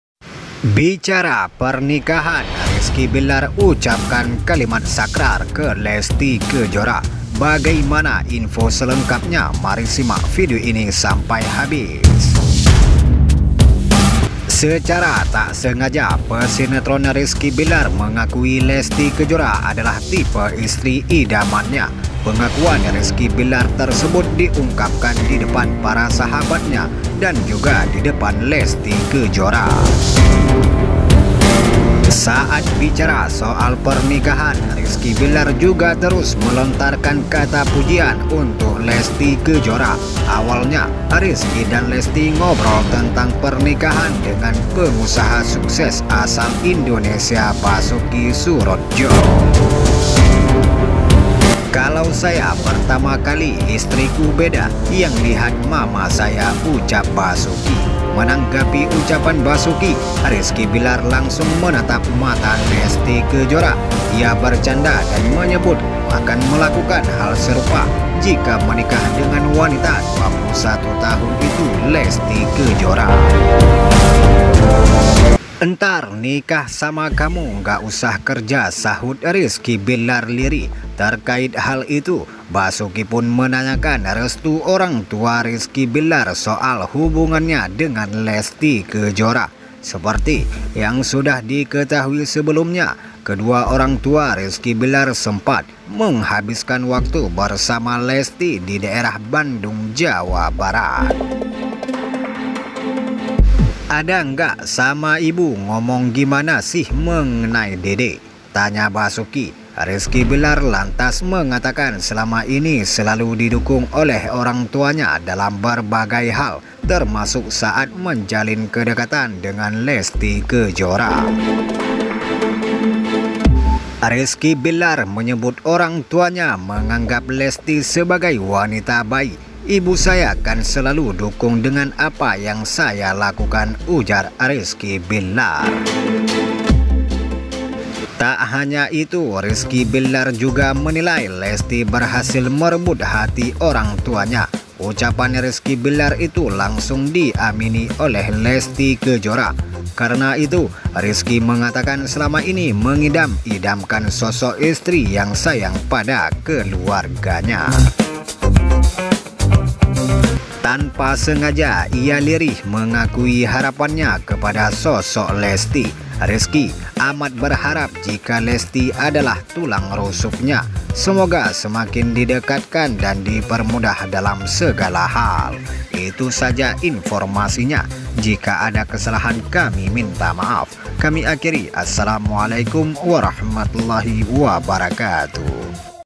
RIZKY BILLAR UCAPKAN KALIMAT SAKRAR INI, BERJANJI INGIN SEGERA NIKAHI LESTY
RIZKY BILLAR UCAPKAN KALIMAT SAKRAR YANG INGIN SEGERA NIKAHI LESTY.